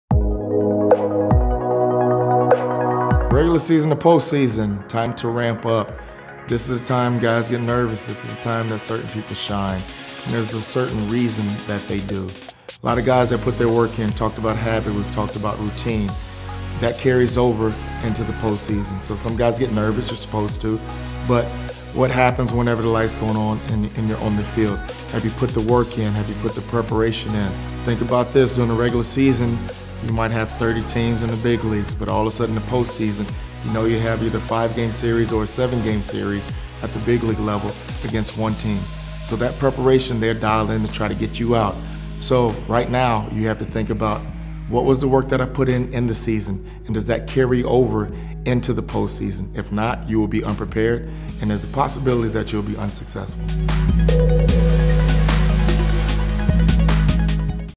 Marlon Byrd on Transitioning Regular Season Success into the Postseason
Making a deep run toward a championship takes plenty of preparation. The work you put in during the regular season can heavily dictate your postseason success. Listen as former baseball pro Marlon Byrd outlines how to transfer regular season success into the postseason. Use these baseball Pro Tips and make sure you’re ready for your playoff push.

PRODUCTS
Shop DICK’S Sporting Goods for your favorite baseball gear.

SOCIAL
Become a part of the DICK’S Sporting Goods community: